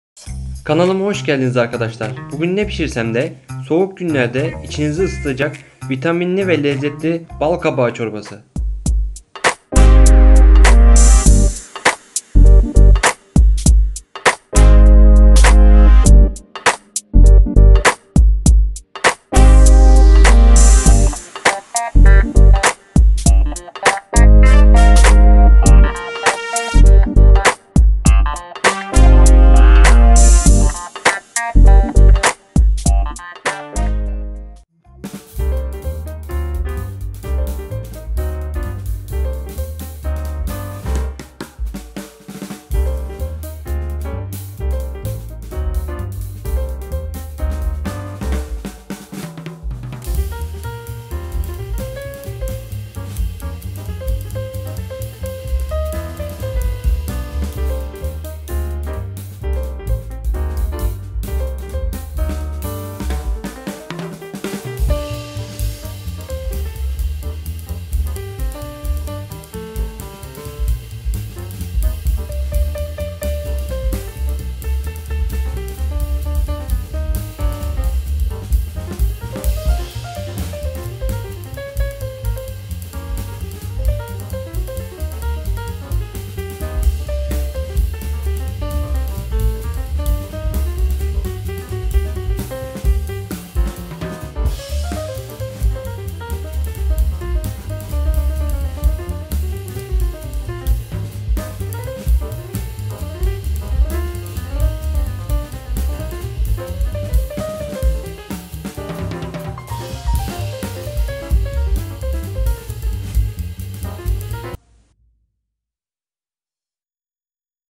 Vitamin Deposu Bal Kabağı Çorbası
Herkese Merhaba.Bugün ne pişirsemde soğuk kış günlerinde içinizi ısıtacak vitamin deposu bal kabağı çorbası ile karşınızdayım.Çocuklarınızın severek içebileceği harika lezzetiyle,altın sarısı görünmüyle bal kabağı çorbasını mutlaka denemelisiniz.

Malzemeler
1/4 bal kabağı
1 adet soğan
4 diş sarımsak
2 adet patates
1su bardağı süt
tuz
Tencereye boyutuna göre su